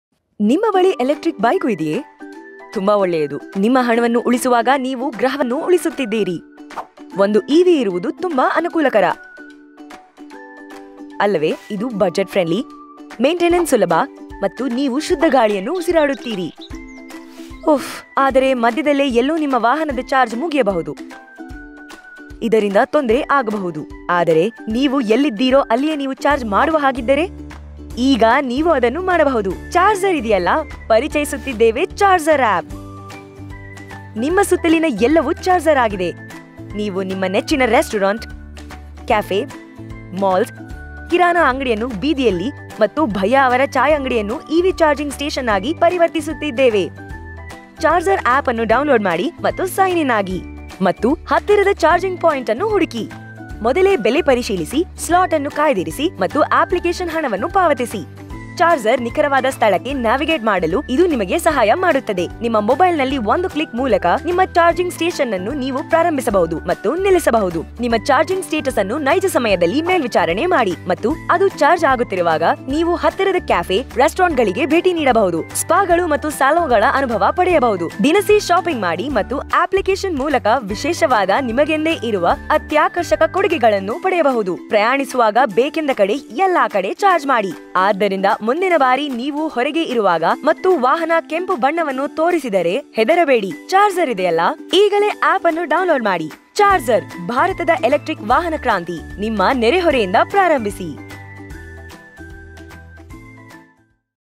2D App Explainer Video | Charzer Kannada | Doodle Mango
Installing charging spots for electric vehicles was a great idea for our client. But, they had a hard time explaining. Fortunately, we, DoodleMango took up the chance and made them a clear explainer video to overcome the process.